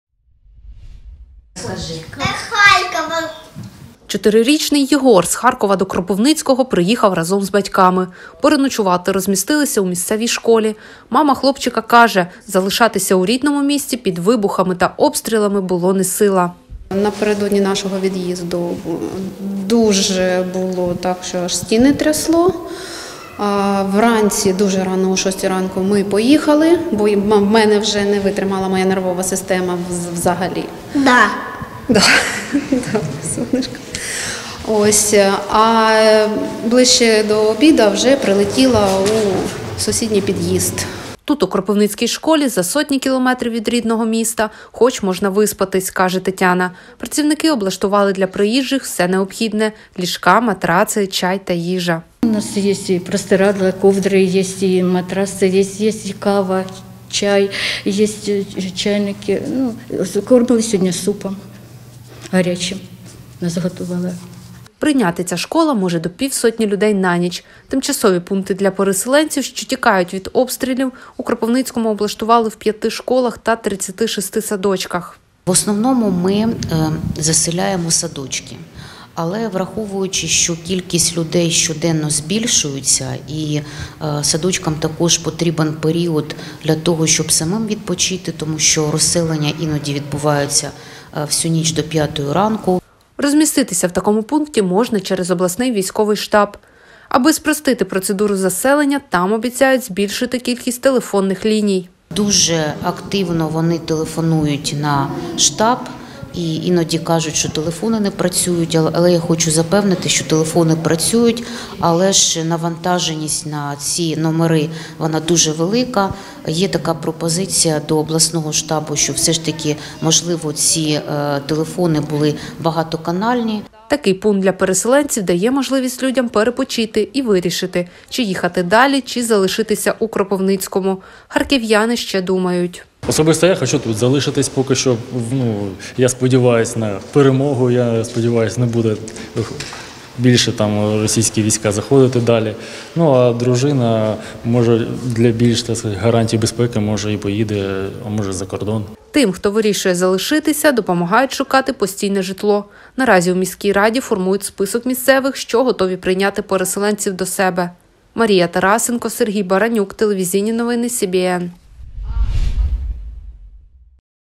Кропивницький продовжує приймати переселенців | телеканал Вітер
Тимчасові пункти для переселенців у Кропивницькому облаштували в 5 школах та 36 садочках. Тут люди можуть переночувати, відпочити і вирішити - чи їхати далі, чи залишитись в місті.